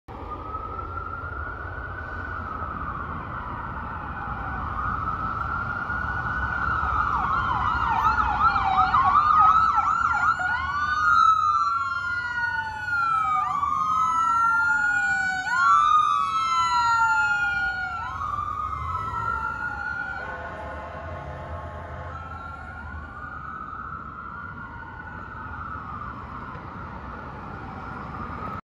Idiot driver doesn’t move for ambulance in Calgary
SUV driver takes too long to move and ambulance is not patient enough to wait.

Yield to emergency vehicles!!!